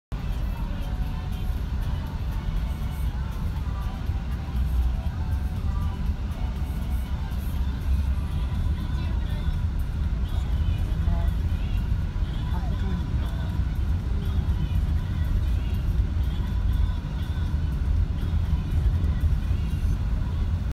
白川鄉的特大風雪中！
白川鄉遇上幾十年來最大的風雪！